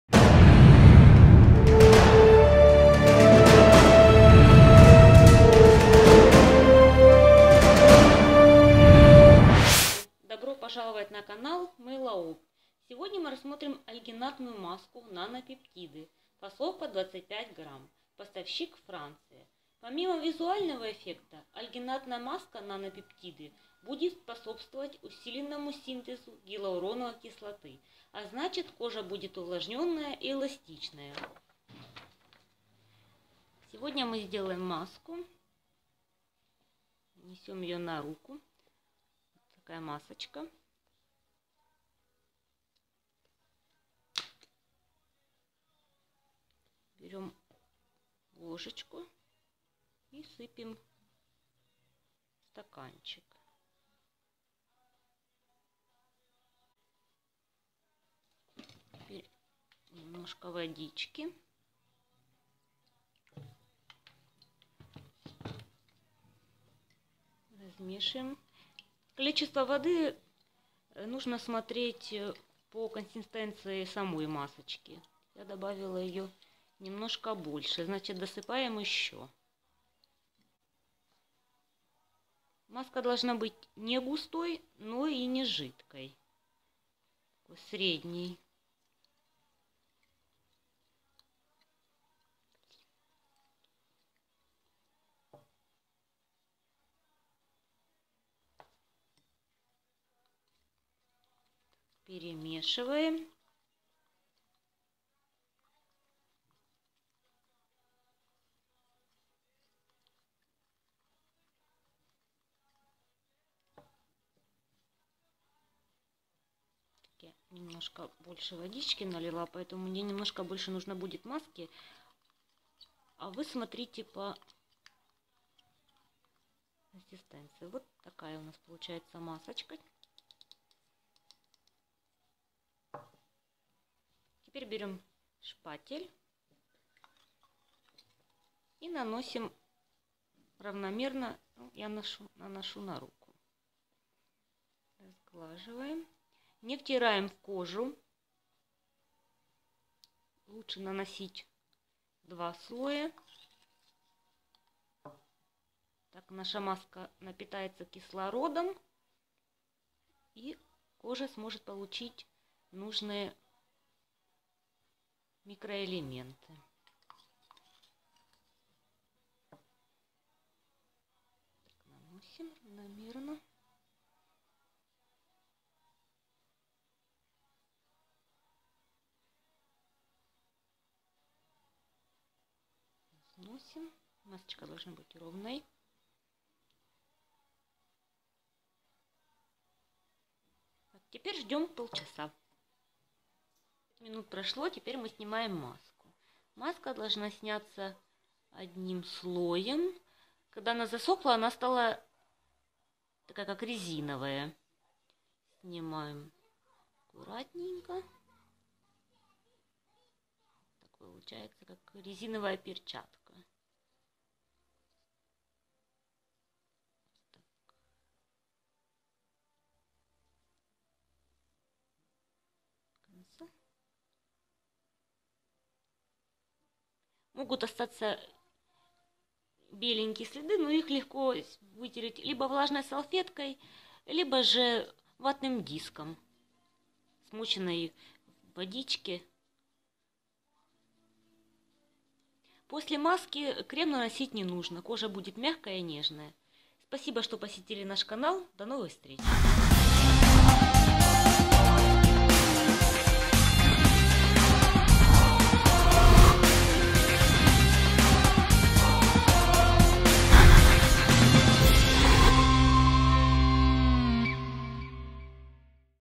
DIY: Мастер-класс// Альгинатная маска// Нано пептиды для кожи
Мастер-класс Альгинатная маска с нано пептидами от Мыло-опт

Сегодня мы с вами наши постоянные Ютубозрители увидем как в домашних условиях можно очень легко сделать себе процедуру альгинатная маска.
Это очень глубокое увлажнение кожи, это и легкая подтяжка, восстановления после тяжелого рабочего дня. Маска с нано пептидами насытит кожу витаминами, которые она теряет каждый день! рекомендую !!!